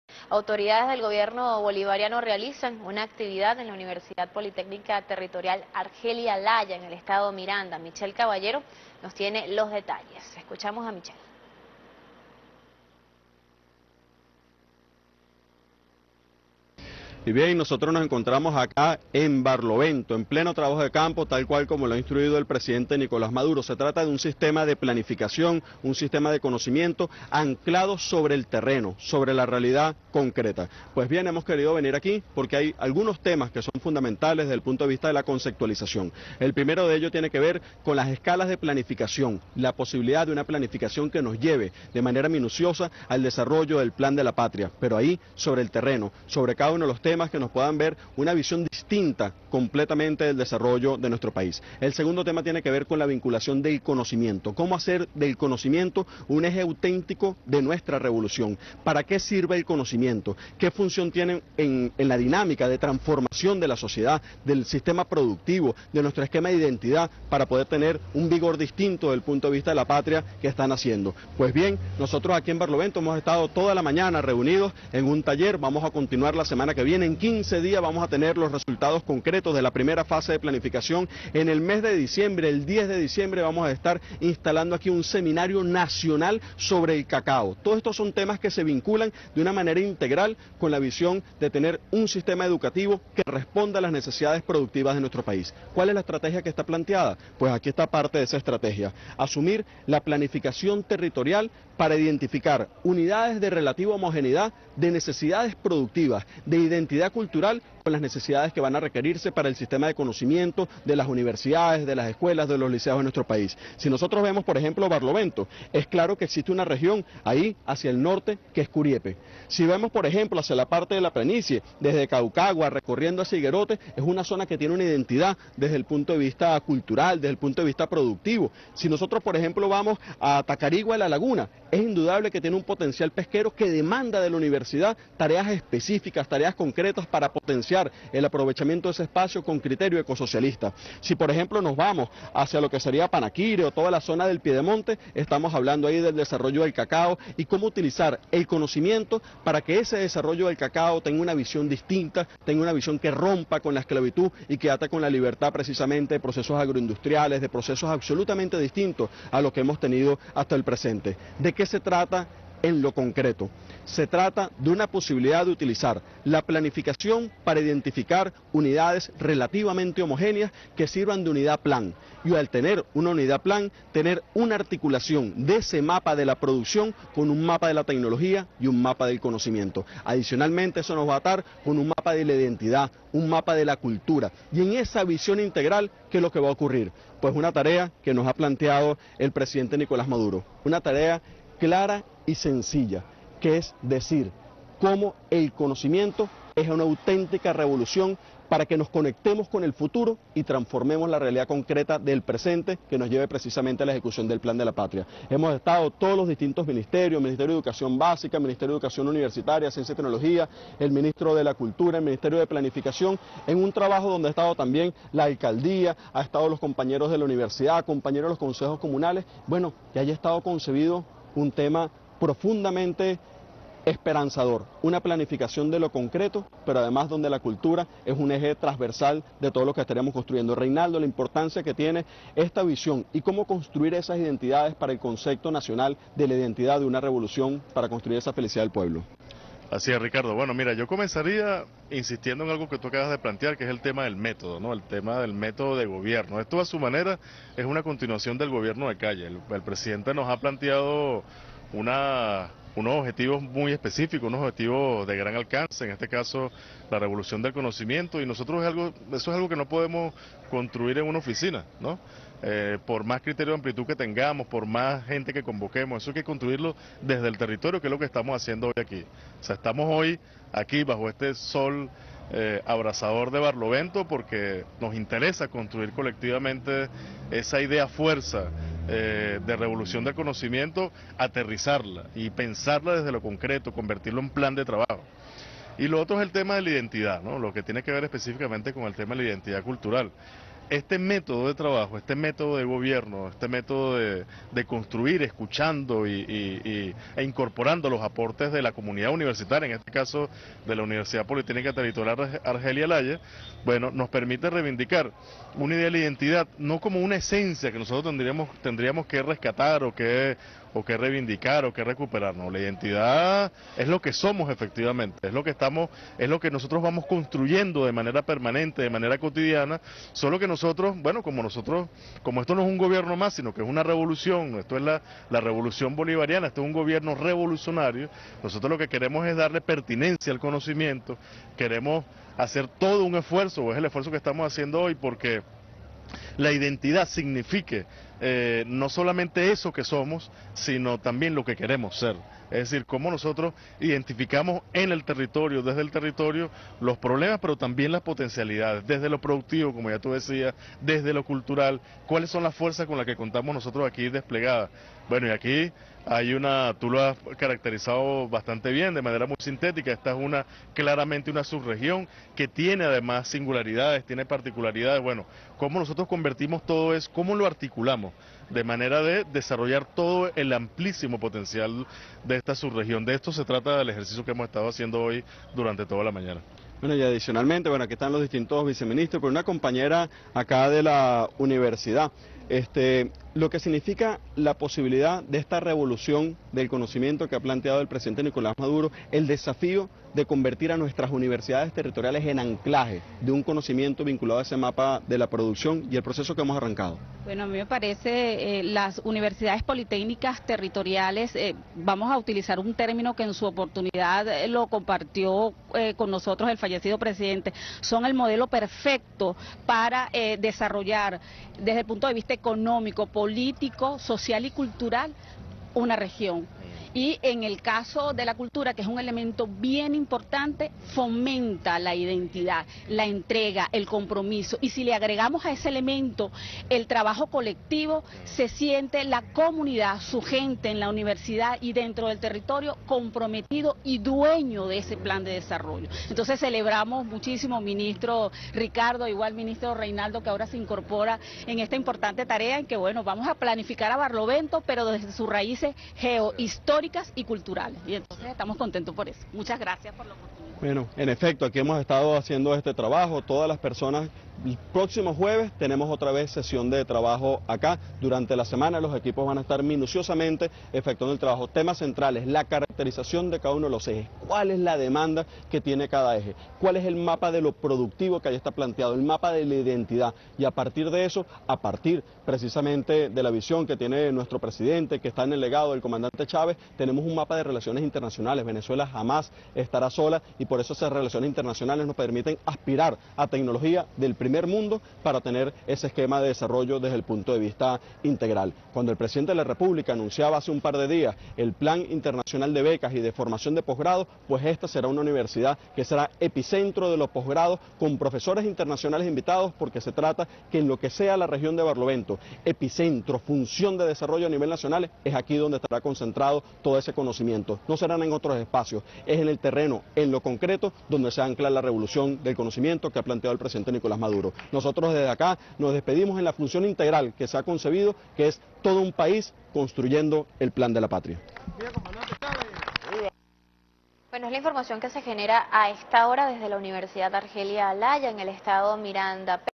UPT Argelia Laya de Barlovento: Ricardo Meléndez y el ministro de Cultura Reinaldo Iturriza
El vicepresidente de Planificación y Conocimiento, Ricardo Meléndez, y el ministro de Cultura, Reinaldo Iturriza, visitaron este 30 de octubre la UPT Argelia Laya en Barlovento